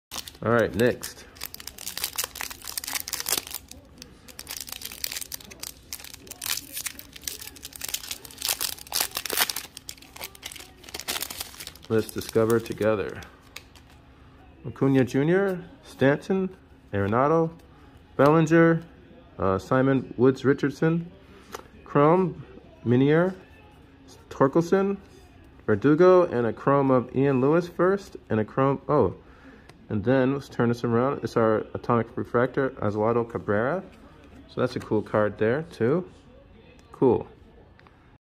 2022 Bowman Baseball pack 10: Atomic refractor of....
I visited Hobby Korea and had to grab another 2022 Bowman Baseball. They are half the price and probably going to run out soon. You can still get some great firsts out of this product, so if you can grab one for cheap, do so.